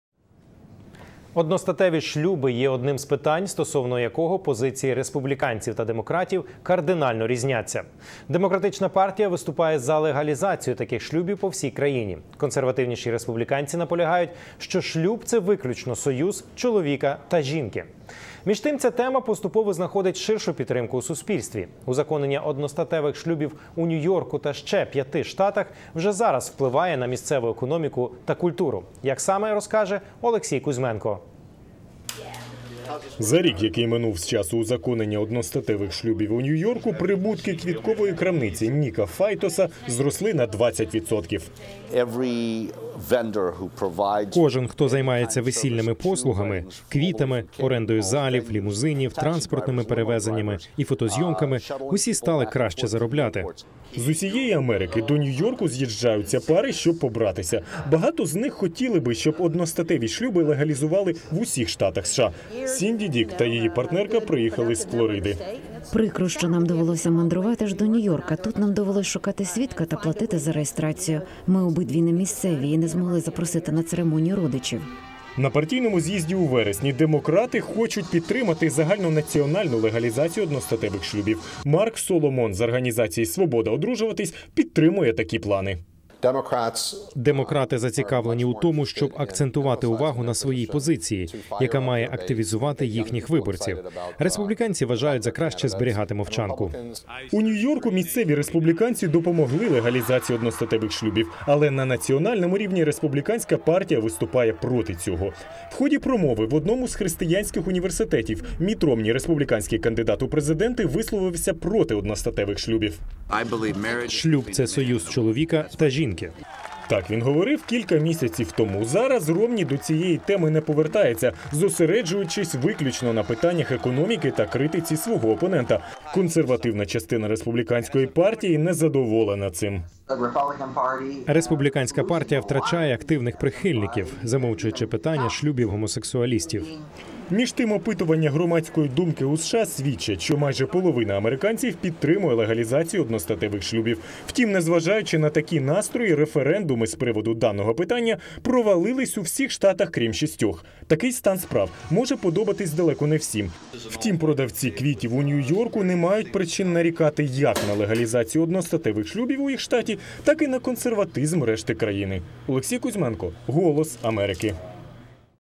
Питання одностатевих шлюбів у виборчій кампанії у США
На партійному з'їзді у вересні демократи хочуть підтримати загальнонаціональну легалізацію одностатевих шлюбів.